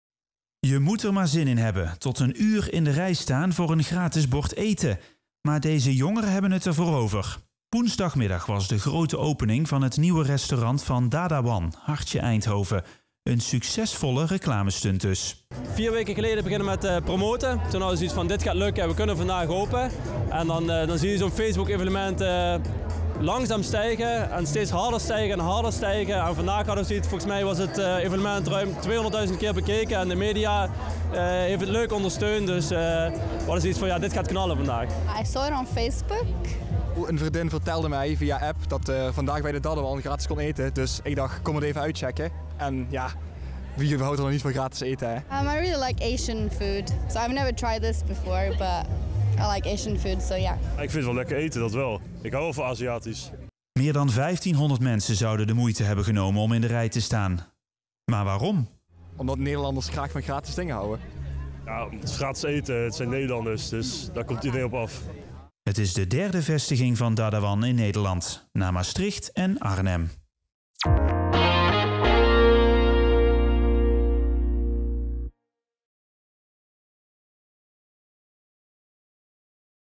In de rij voor een gratis bord eten bij Dadawan
In het centrum van Eindhoven is woensdagmiddag een lange rij ontstaan voor winkelcentrum Heuvel.